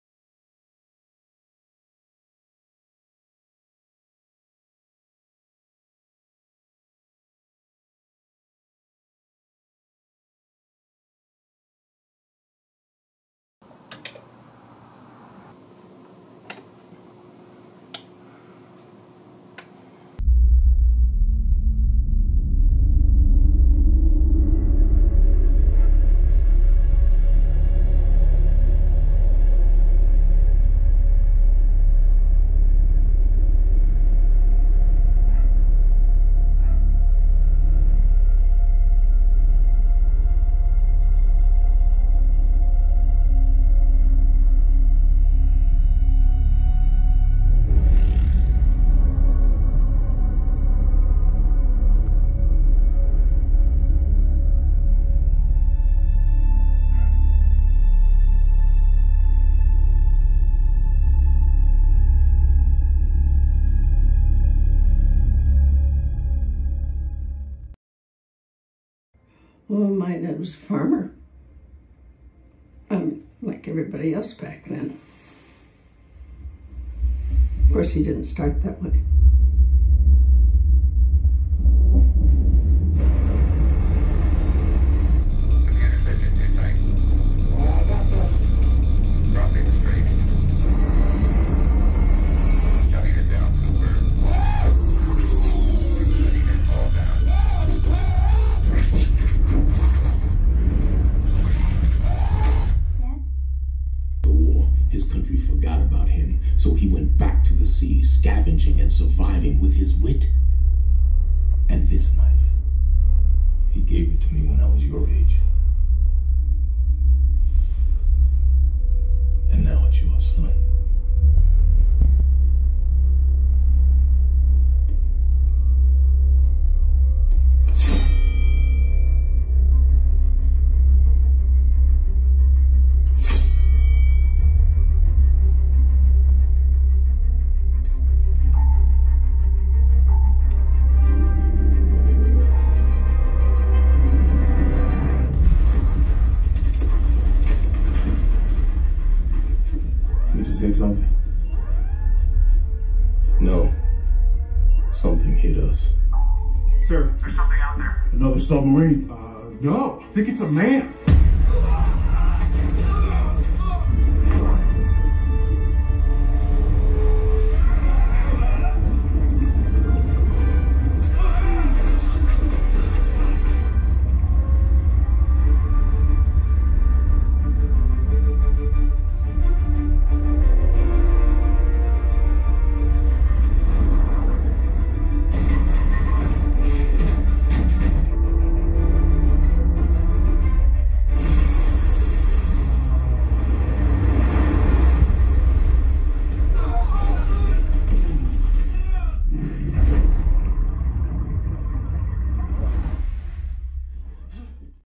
4 Car Subwoofers in Home Surround Sound
Using Car subs and yeah they sound amazing.  For what I paid they provide plenty...and I mean plenty of boom.